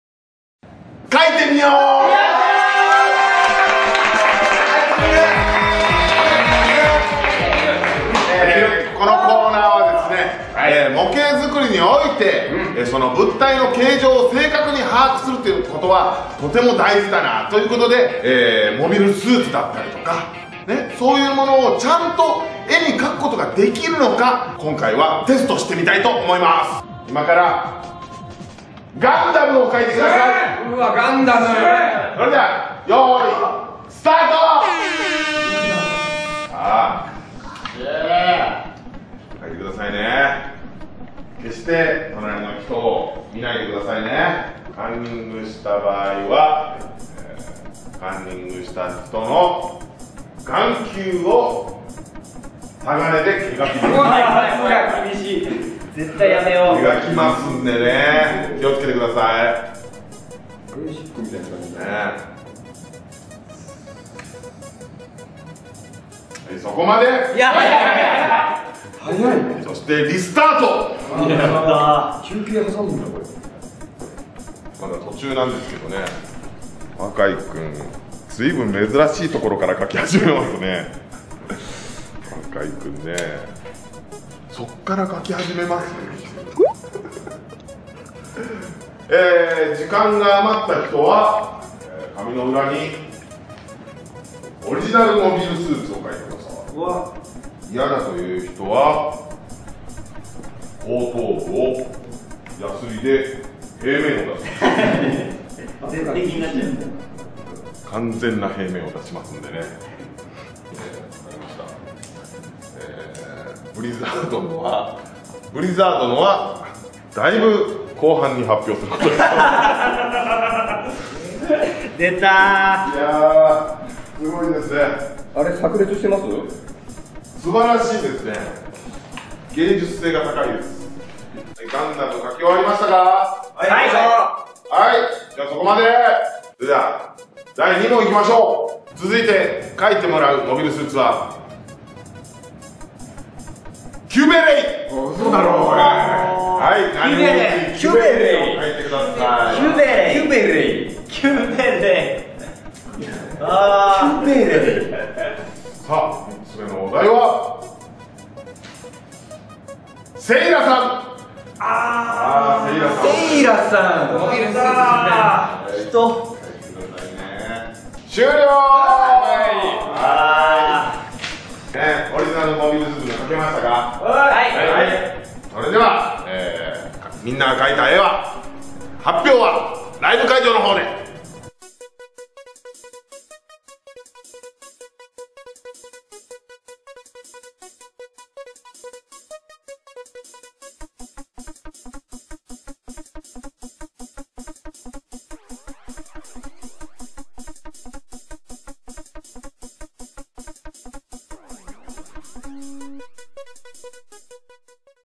【絵心チェック】 吉本プラモデル部 描いてみよう! 吉本プラモデル部ライブPG ～灼熱のヒートペン編～
吉本プラモデル部チャンネルです!!

吉本プラモデル部ライブPG　～灼熱のヒートペン編～

吉本プラモデル部部員の画力を抜き打ちでチェック!!

こちらのサイトで最新情報をチェック!!

編集担当じゃじゃーん菊池
じゃじゃーん菊池GAMEチャンネル